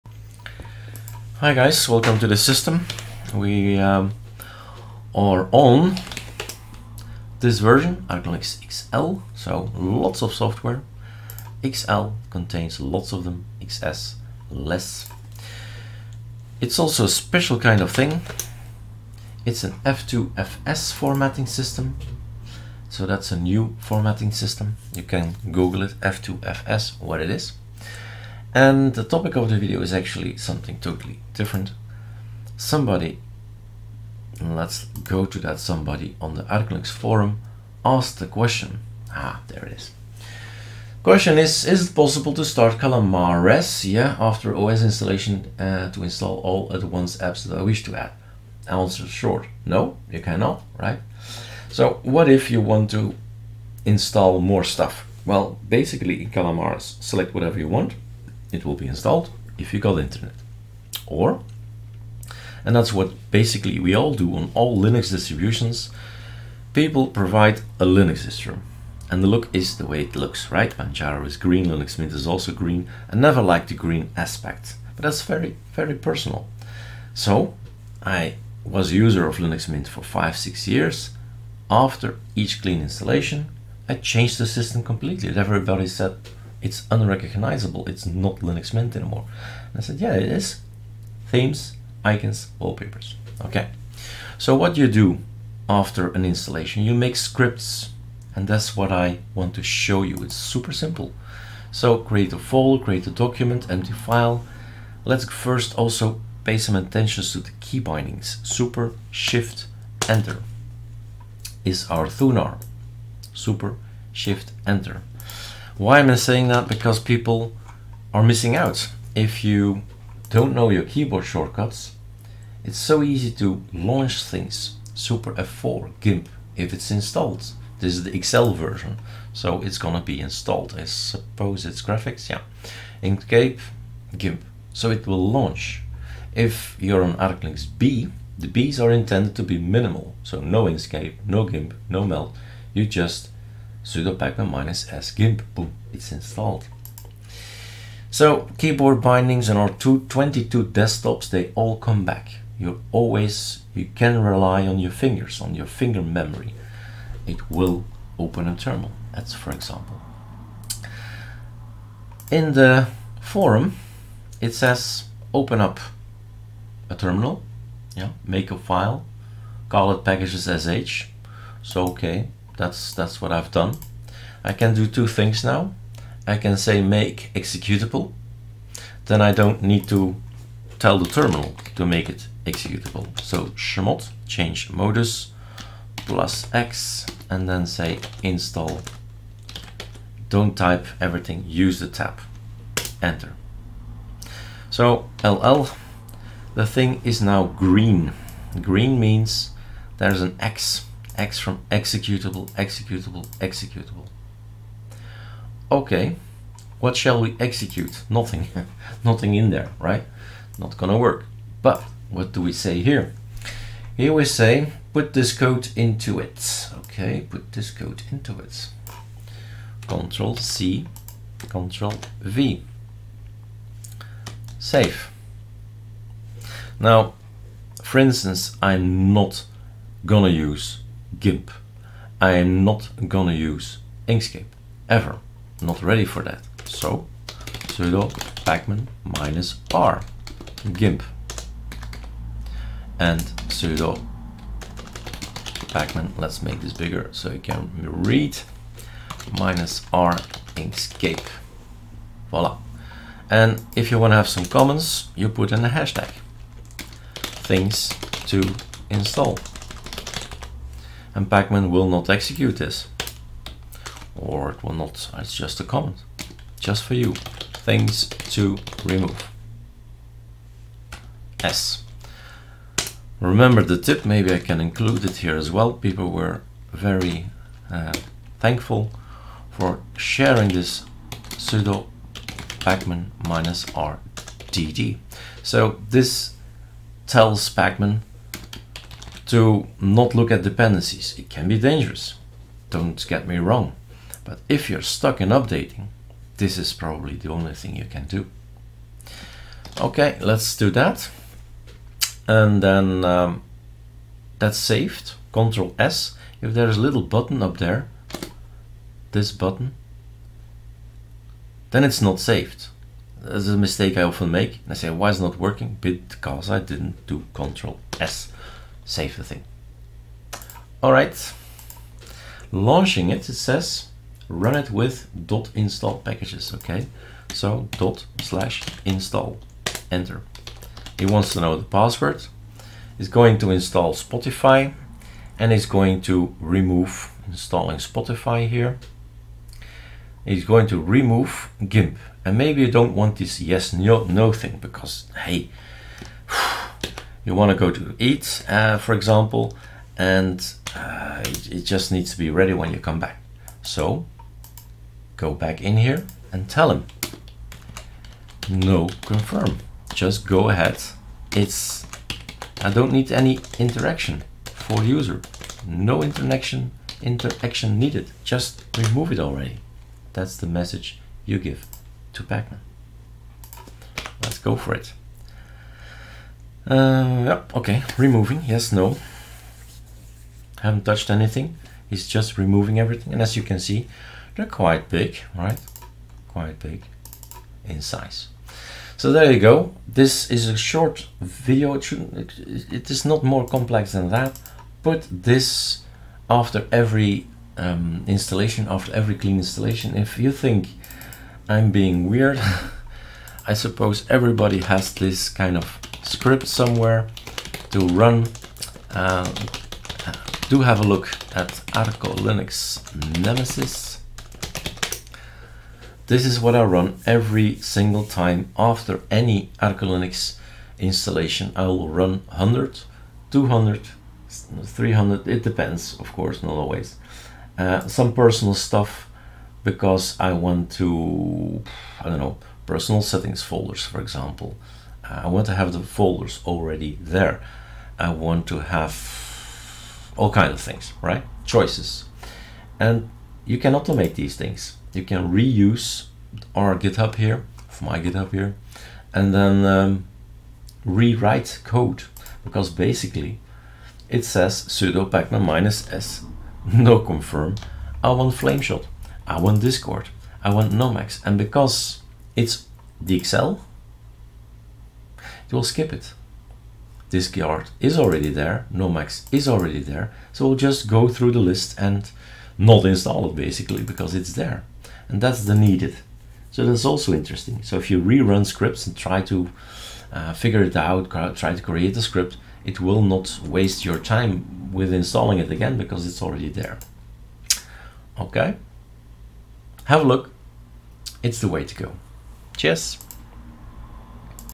ArcoLinux : 2030 how to make your own installation script - what to do after a clean install
A tutorial created for this article
NO ARTICLE